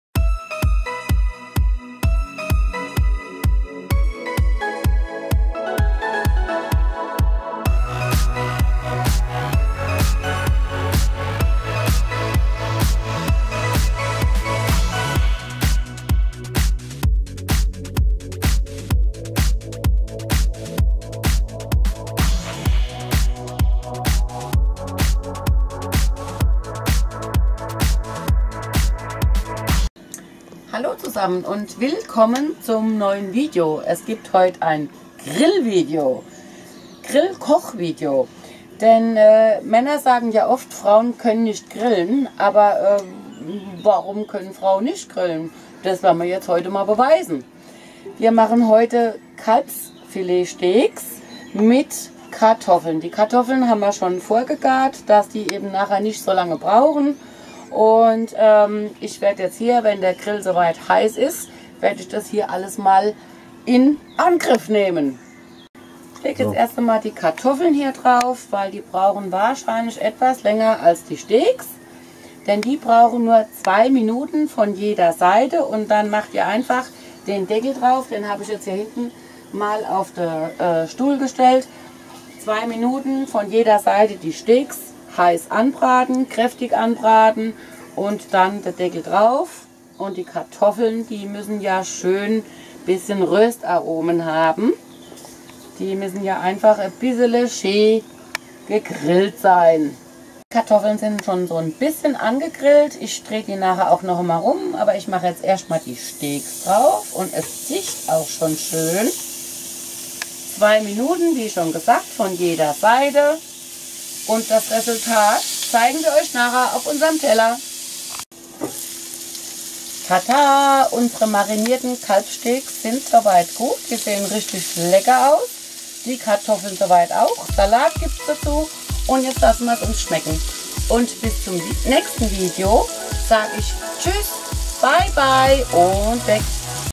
Cadac Citi Chef 40 im Einsatz
auch wir Camping-Frauen können Grillen.
marinierte Kalbssteacks an Grillkartoffeln.

carmen&chris on tour
camping gas grill
camping grill
gas grill
cadac grill
cadac citi chef 40